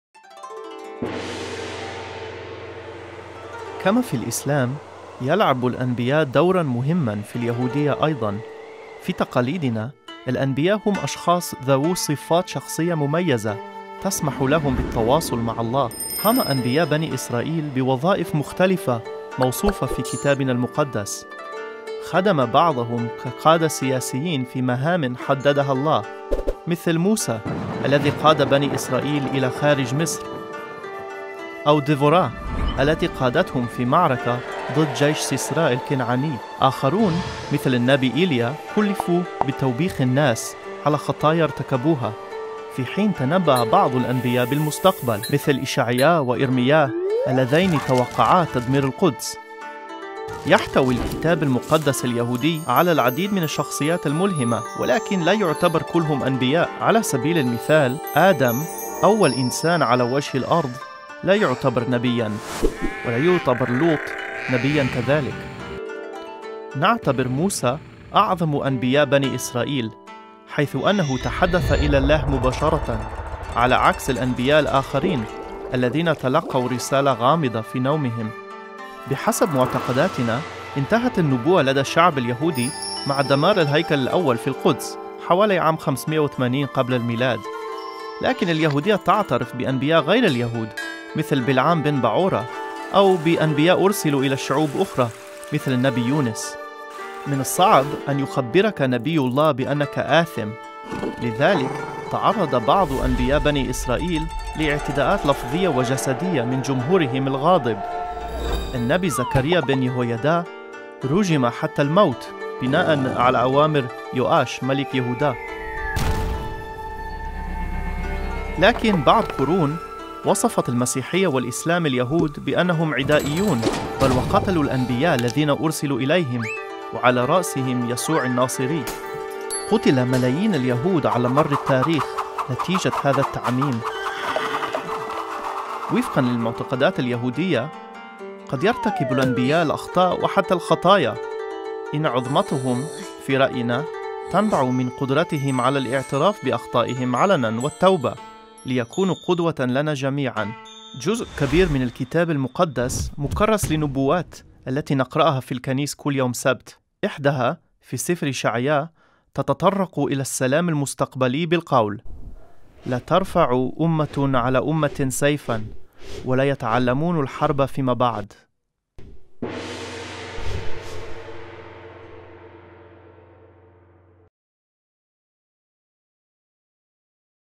الأنبياء في اليهودية
كما هو الحال في الإسلام، الأنبياء يلعبون دور أساسي في اليهودية أيضًا. من القيادة السياسية إلى التنبؤ بالمستقبل، اكتشفوا أهمية الأنبياء لليهود.